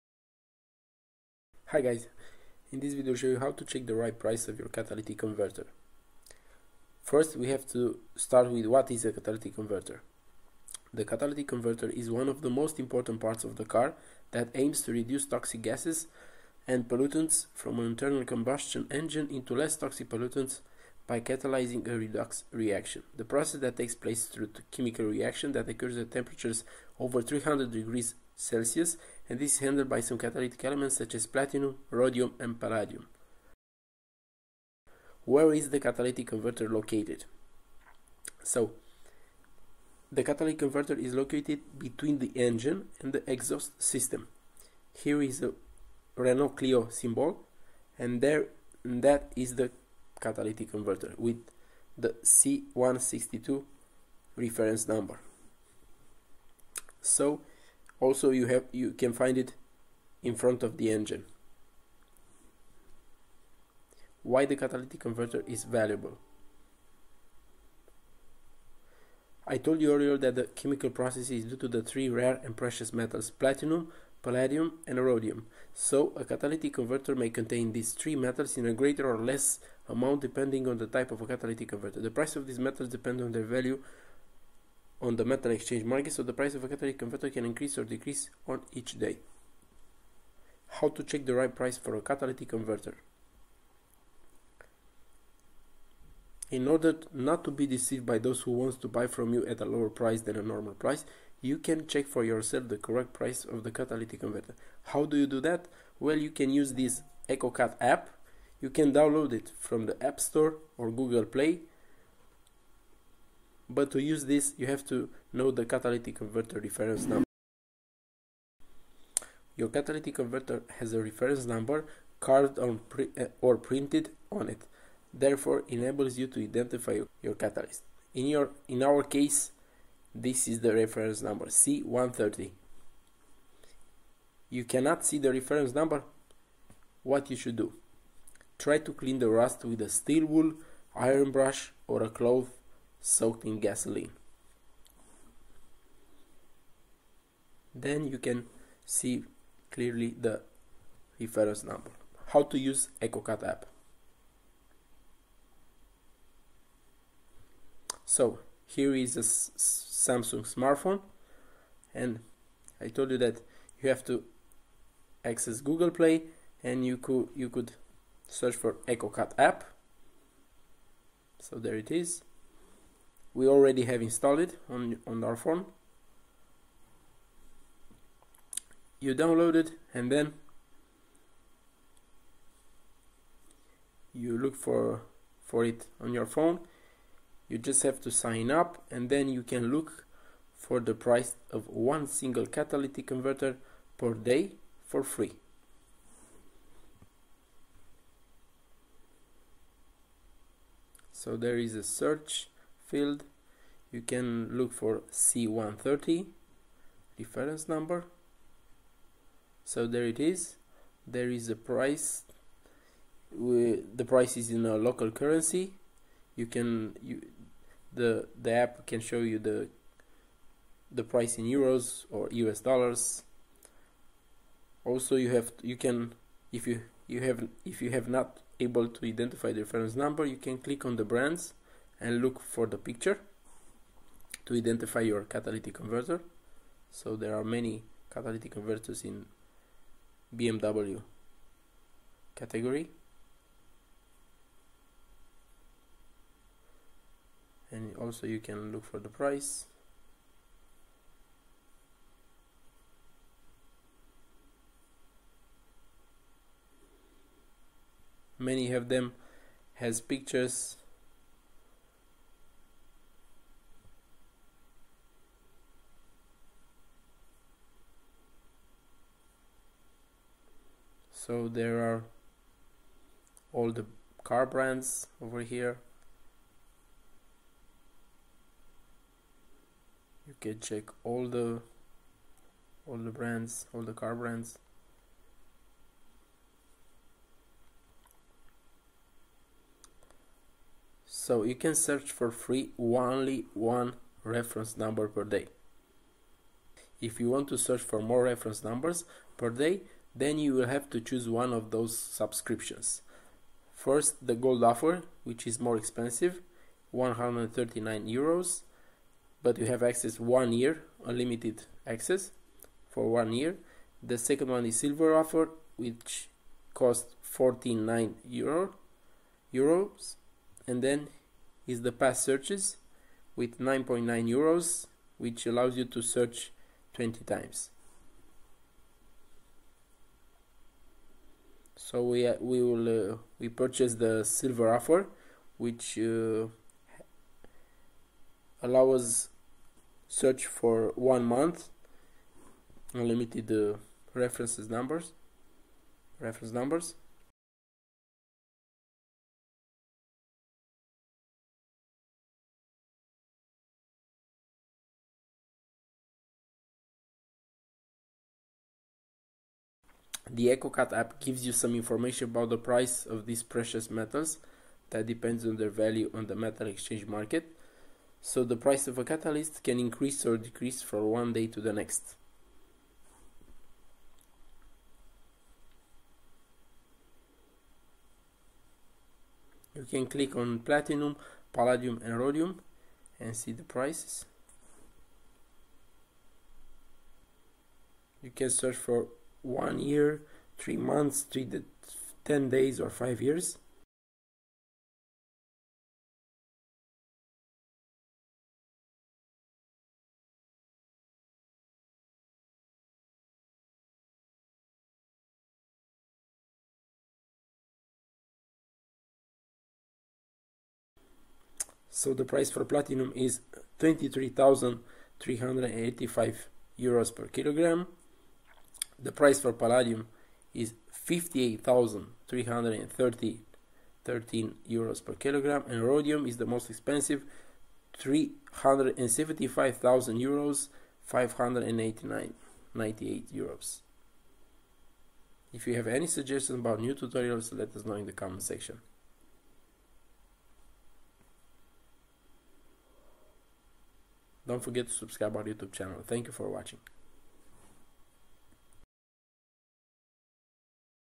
TUTORIAL: How to check the RIGHT PRICE of your catalytic converter (from your car)
In order not to be deceived by those who want to buy from you at a lower price than the normal price, you can check for yourself the correct price of the catalytic converter using Eco Cat App.

The Eco Cat App give some information about the price of these metals depends on their value on the metal exchange market, so the price of a catalyst can increase or decrease from one day to the next.

All about money that makes money. Change the game! 8 steps to know everything you need to know to get there!